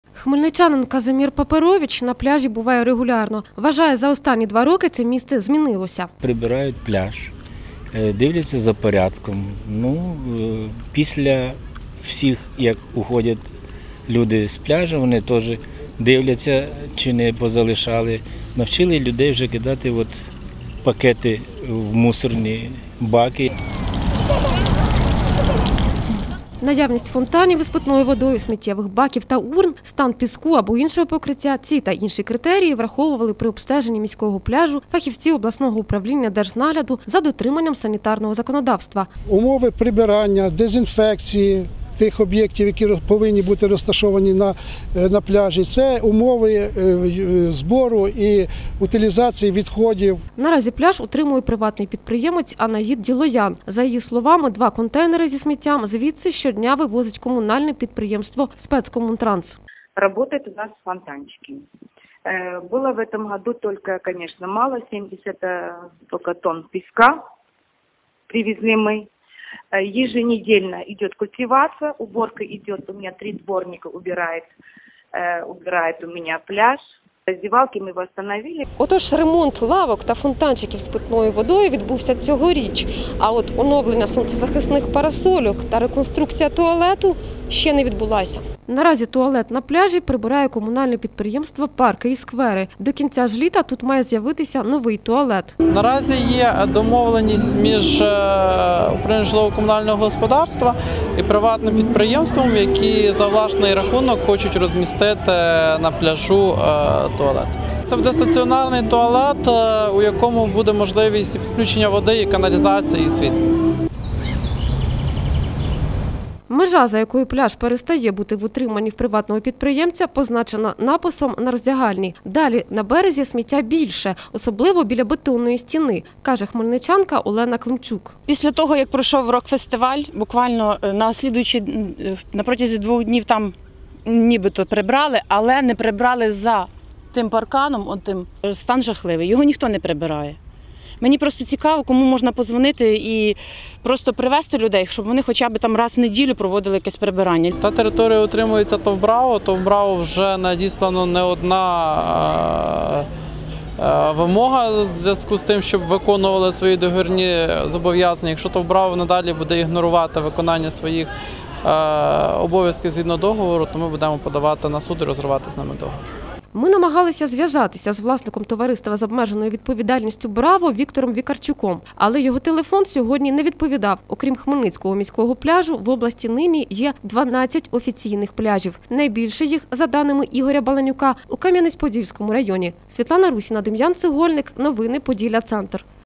Хмельницький міський пляж – єдине офіційно затверджене місце для купання в обласному центрі
Хмельницький міський пляж – єдине офіційно затверджене місце для купання в обласному центрі. Бо відповідає санітарним вимогам. Про це повідомив начальник управління держнагляду за дотриманням санітарного законодавства обласного управління Держпродспоживслужби Ігор Баланюк. За його словами, під час обстеження фахівці управління враховували стан водного плеса і піску, наявність питної води. Перевіряли умови прибирання сміття і туалетів.